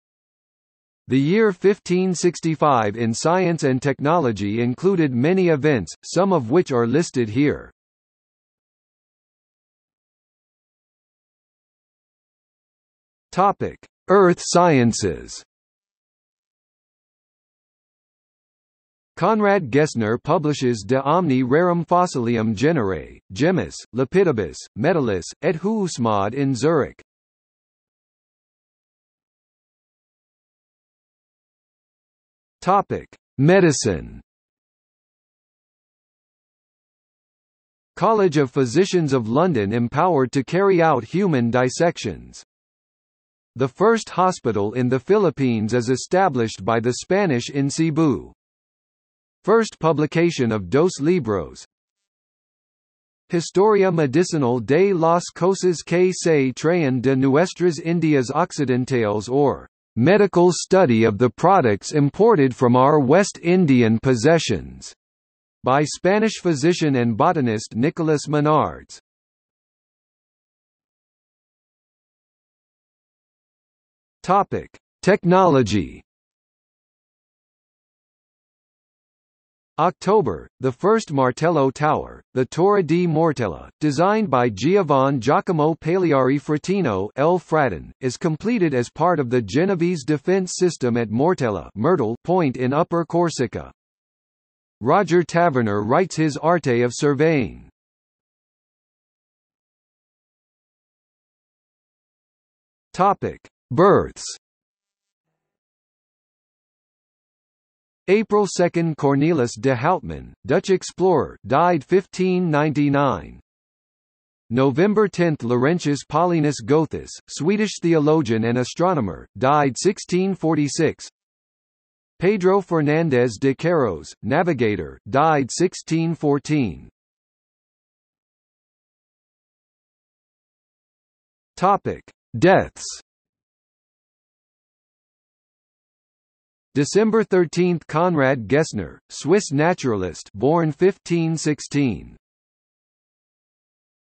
1565 in science | Wikipedia audio article
00:00:09 1 Earth sciences
00:00:29 2 Medicine
00:01:10 3 Technology
00:01:39 4 Births
00:02:07 5 Deaths

- Socrates

SUMMARY
The year 1565 in science and technology included many events, some of which are listed here.